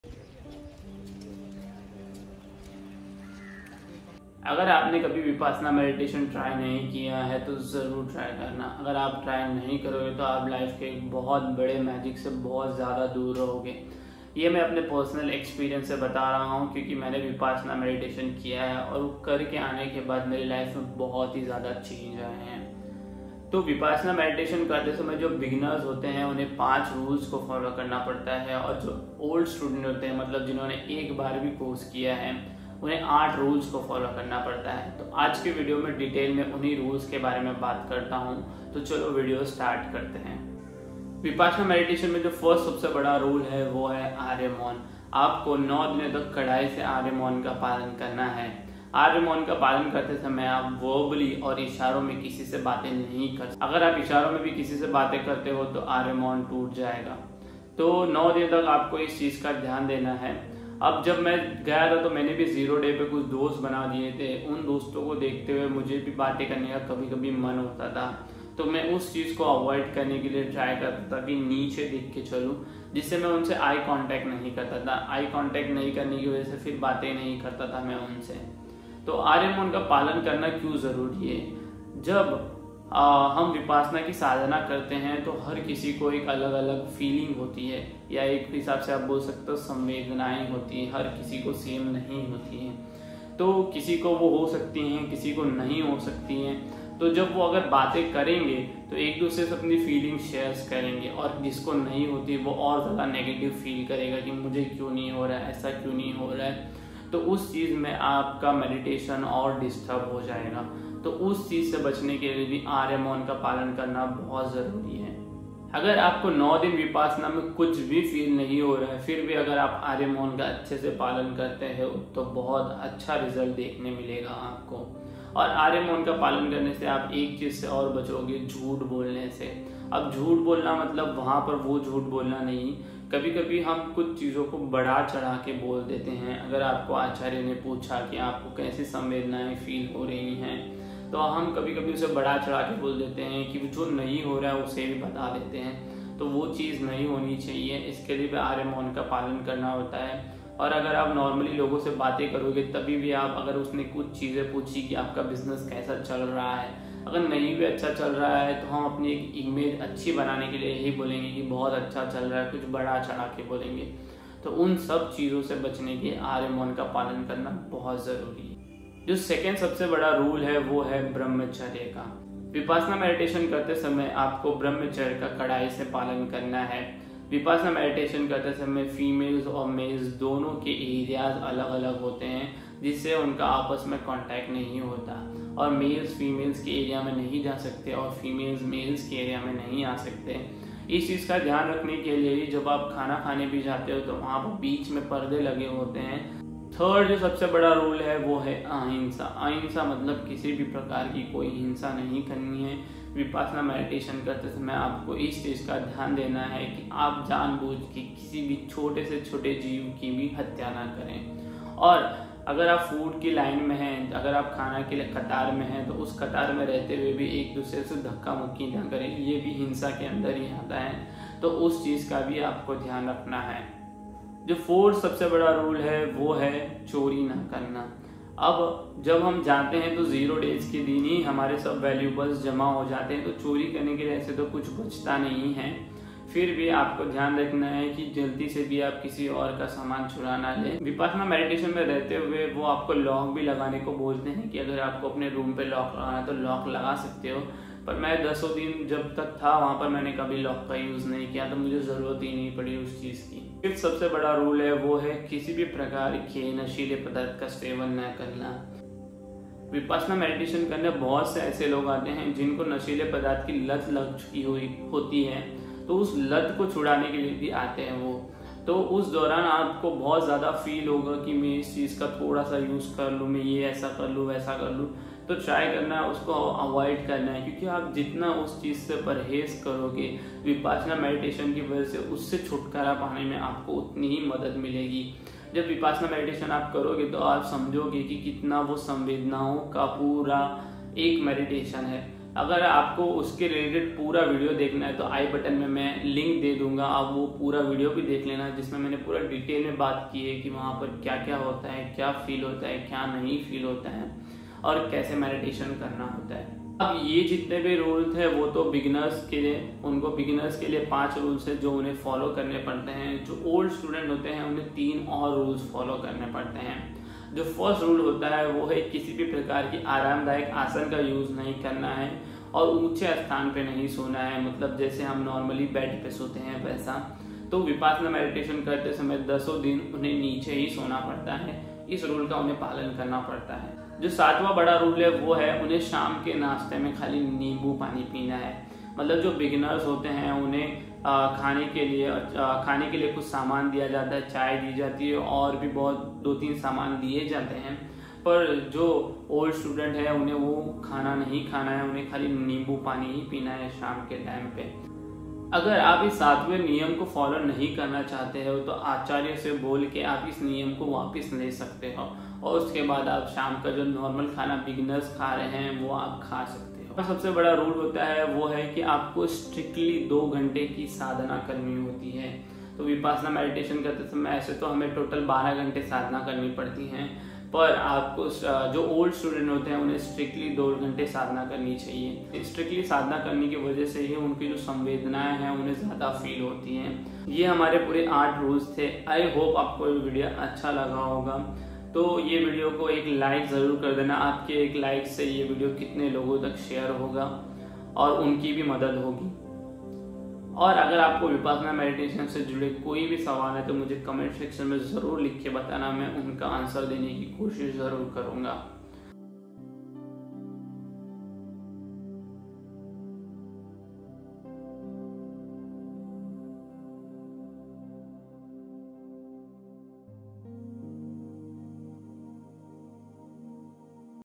Vipassana Meditation: 5 Essential Rules for New and Experienced Practitioners
In this insightful video, we delve into the fundamental principles of vipassana meditation offering a comprehensive guide for both new and seasoned practitioners. Discover the transformative power of this ancient technique as we outline the 5 essential rules for beginners known as the Aaryamon brahmacharya asteya (non-stealing), ahimsa (non-violence), and abstinence from intoxicants. Moreover, we unravel 8 additional rules specifically designed for experienced meditators. Join us on this enlightening journey and unlock the profound wisdom of Vipassana, paving the way to inner peace and personal growth. Whether you are new to meditation or have been practicing for years, this video is a must-watch for anyone seeking a deeper understanding of Vipassana's guiding principles.

 Introduction: 0:00 - 0:40
   - Briefly introduce the topic of Vipassana meditation and the purpose of the video.

   - Aaryamon: 0:40 - 2:48
   - Brahmacharya: 2:49 - 3:16
   - Asteya (Non-Stealing): 3:44 - 4:13
   - Ahimsa (Non-Violence): 3:17 - 3:43
   - Abstinence from Intoxicants: 4:14 - 5:19

 3 Additional Rules for Old Students: 5:20 - 6:26
   - Rule 1: 5:20 - 5:33
   - Rule 2: 5:34 - 6:06
   - Rule 3: 6:07 - 6:26

conclusion   6:27 - 7:05